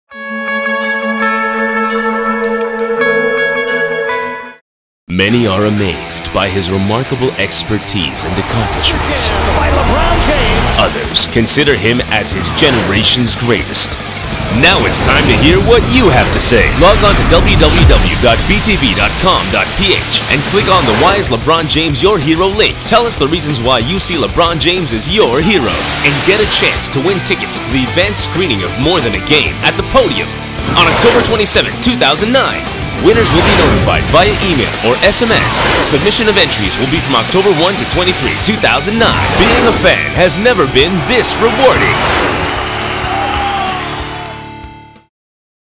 LeBron James Promo Plug 2009
- Sports: Basketball
- Promo Plug for Basketball TV (Solar Entertainment Corporation
- Airing Date: October 8, 2009